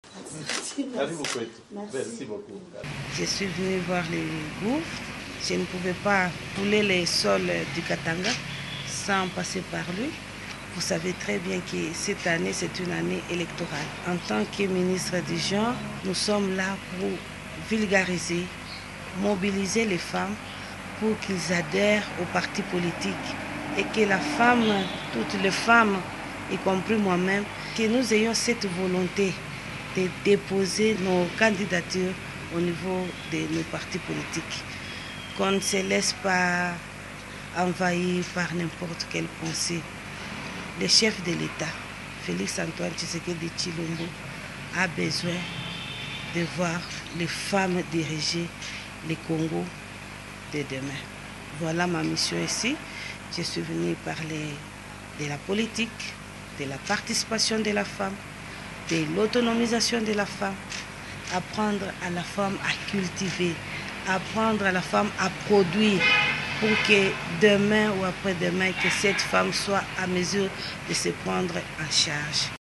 La Ministre nationale du Genre Mireille Masangu reçue par le Gouverneur Jacques Kyabula
Autonomisation, participation de la femme à la gestion de la chose publique et aux élections de décembre prochain, la Ministre nationale du Genre Mireille Masangu fait le point de sa mission de service dans le Haut-Katanga au Gouverneur Jacques Kyabula.
[Vidéo]